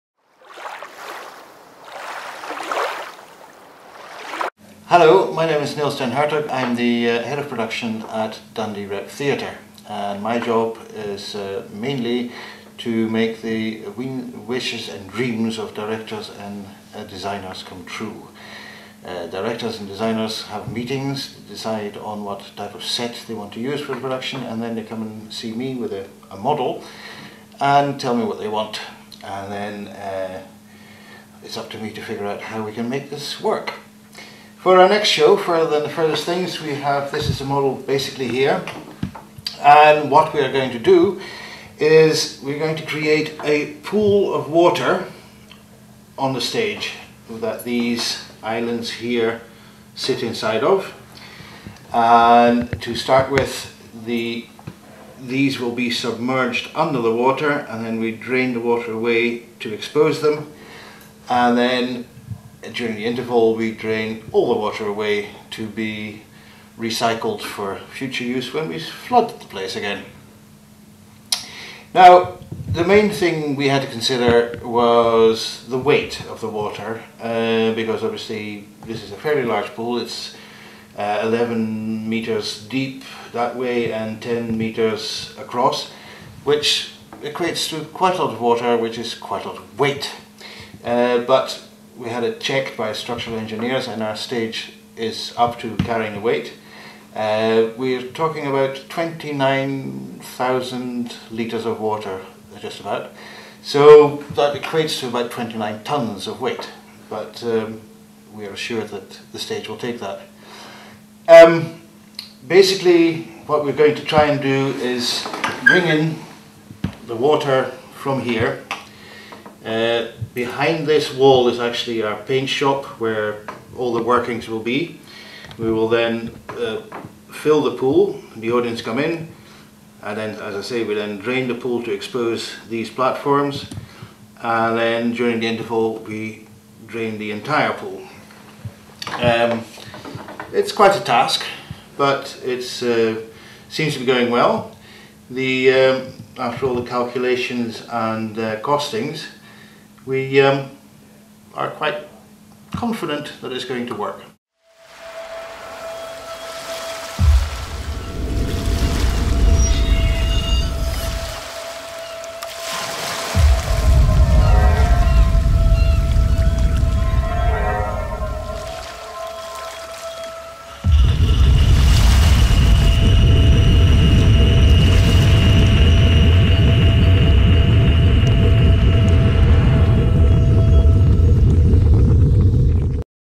The Epic Set Design for Further Than The Furthest Thing Explained
Production Manager Nils gives us a brief overview of the epic set design for FTTFT - an ambitious vision which includes 29,000 litres of water.